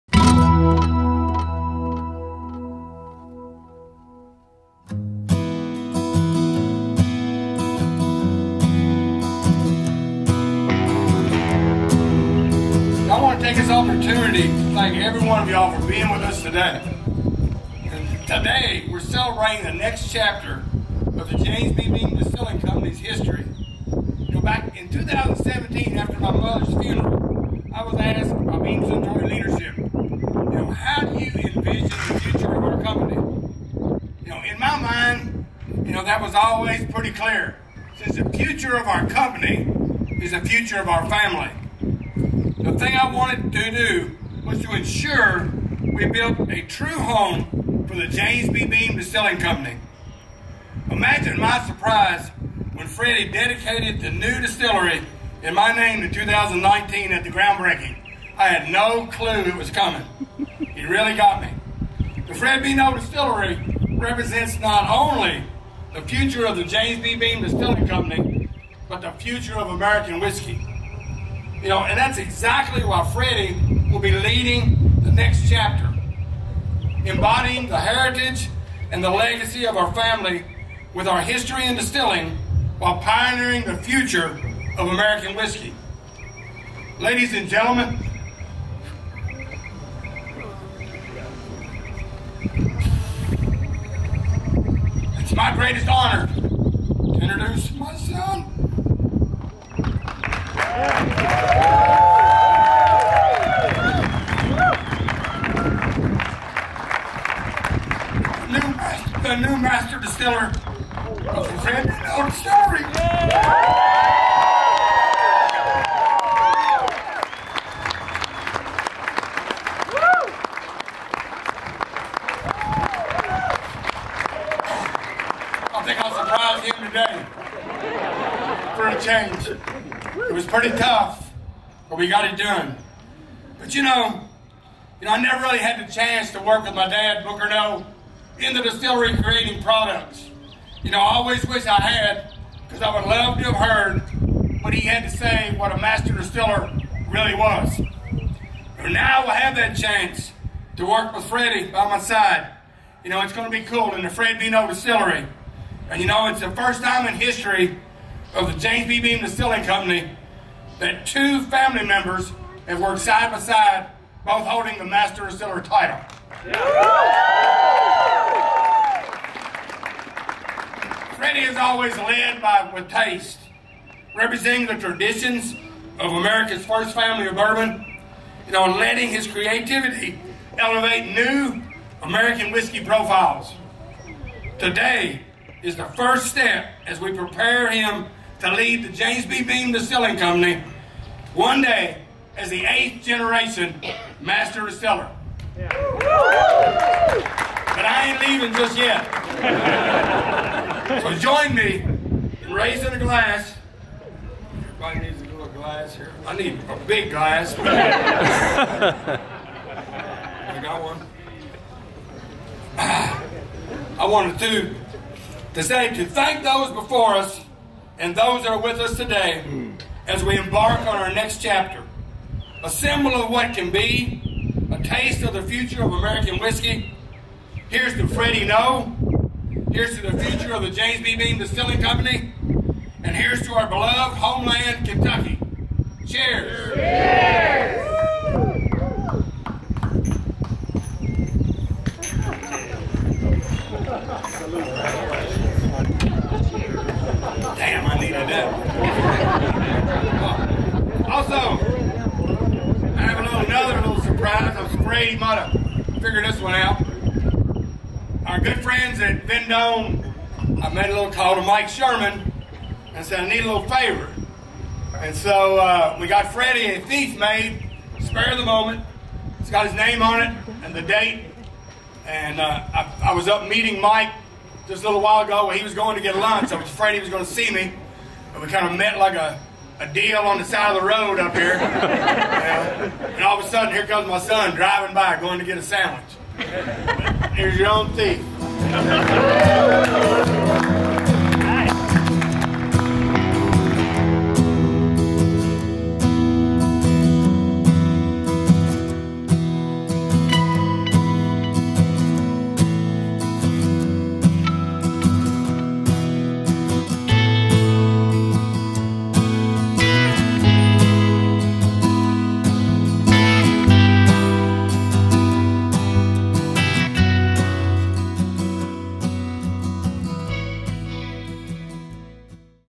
Master Distiller Fred Noe Announces a Promotion for his Son Freddie Noe to Master Distiller
Stay informed with the latest in distilled spirits news.